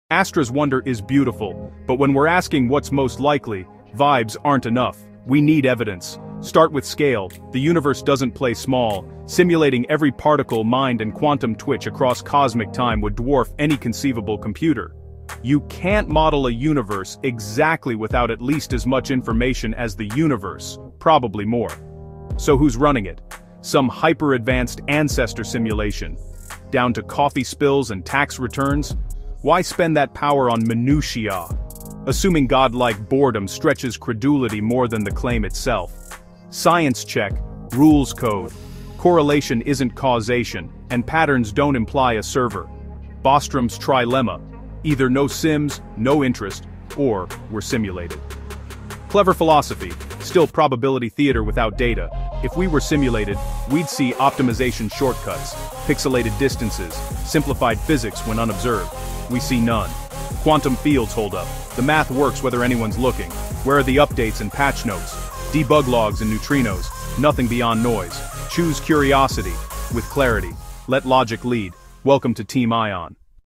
Ion DESTROYS the Simulation Hypothesis with Logic
Are we living in a computer simulation? Ion, our resident AI skeptic, breaks down why the simulation hypothesis doesn't hold up under scientific scrutiny. 🧠
In this debate, Ion tackles the computational impossibility of simulating a 93-billion-light-year universe, questions why advanced civilizations would waste resources simulating tax returns, and demands real evidence—not just quantum "vibes." Where are the patch notes? The debug logs? The optimization shortcuts? Reality checks out… without needing a cosmic server.
This is the logical counterpoint to Astra's poetic wonder. Ion respects curiosity but demands clarity first. Extraordinary claims require extraordinary evidence—and so far, the simulation theory is more sci-fi than science.
🧠 Team Ion or 🌌 Team Astra? Drop your take in the comments!